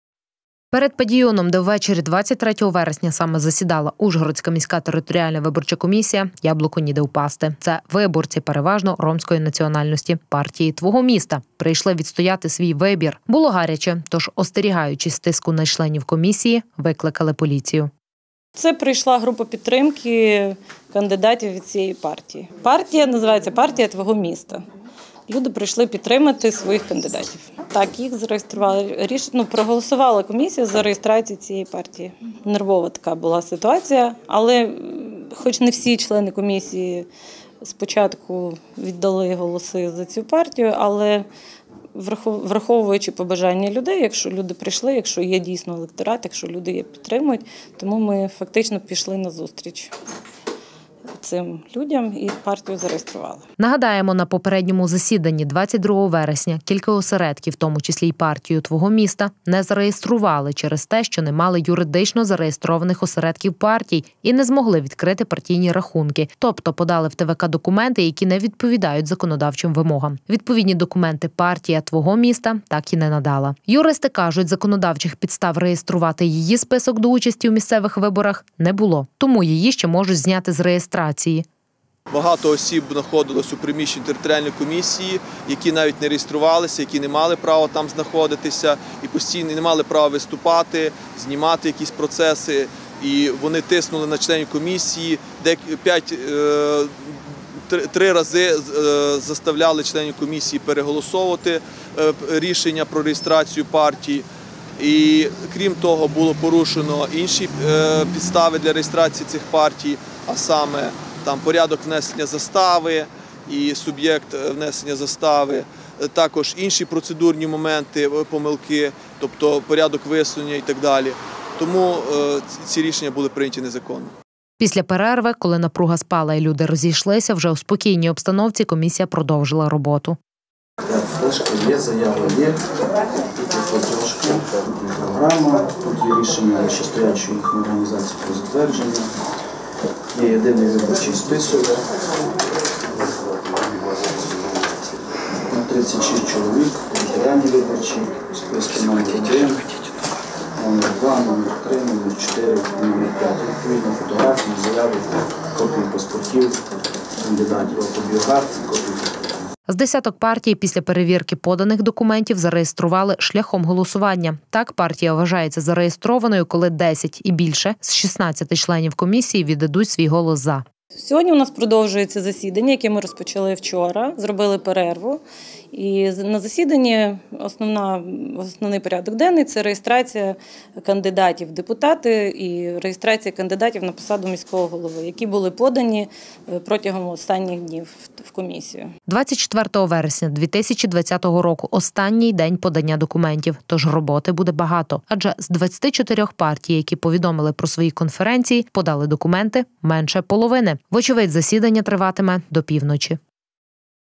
Гаряче засідання. Реєстарція партій
На засіданні Ужгородської міської територіальної виборчої комісії ввечері 23 вересня було гаряче. Фактично – це продовження попереднього засідання, оскільки 22 вересня стало відомо, що частина партій мали юридично зареєстрованих осередків партій і не змогли відкрити партійні рахунки. Тобто подали в ТВК документи, які не відповідають законодавчим вимогам.
Перед ПАДІЮНом, де ввечері 23 вересня саме засідала Ужгородська міська територіальна виборча комісія – яблуку ніде впасти! Це виборці , переважно ромської національності , "Партії твого міста" прийшли відстояти свій вибір! Було гаряче, то ж остерігаючись тиску на членів комісії, викликали поліцію.
Нагадаємо, на попередньому засіданні 22 вересня кілька осередків, у тому числі й «Партію твого міста», не зареєстрували через те, що не мали юридично зареєстрованих осередків партій і не змогли відкрити партійні рахунки. Тобто подали в ТВК документи, які не відповідають законодавчим вимогам. Відповідні документи «партія твого міста» так і не надала! Юристи кажуть, законодавчих підстав реєструвати її список до участі у місцевих виборах не було, тому її ще можуть зняти з реєстрації.
Після перерви, коли напруга спала і люди розійшлися,  вже у спокійній обстановці комісія продовжила роботу. З десяток партій після перевірки поданих документів зареєстрували шляхом голосування. Так, партія вважається зареєстрованою, коли 10 і більше з 16 членів комісії віддадуть свій голос «За». 24 вересня 2020 року – останній день подання документів, то ж роботи буде багато - адже з  24 партій, які повідомили про свої конференції, подали документи менше половини. Вочевидь  засідання триватиме до півночі.